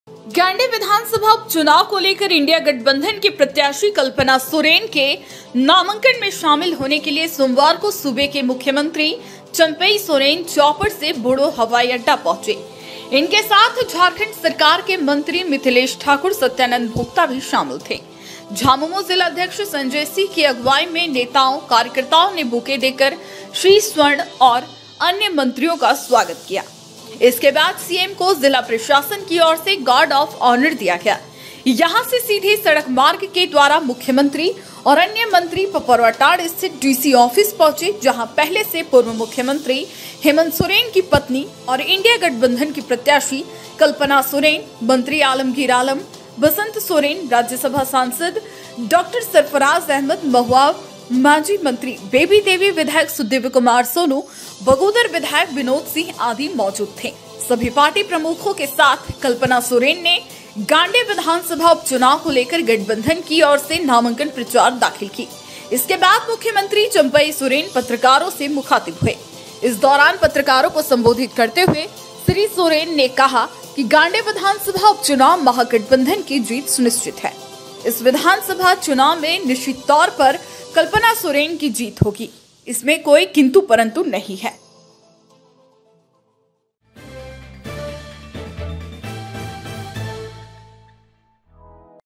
कल्पना सोरेन के नामांकन में शामिल होने के लिए रांची से गिरिडीह पहुंचे मुख्यमंत्री चंपाई सोरेन
कल्पना सोरेन के नामांकन में शामिल होने के लिए रांची से गिरिडीह पहुंचे मुख्यमंत्री चंपाई सोरेन समेत अन्य मंत्री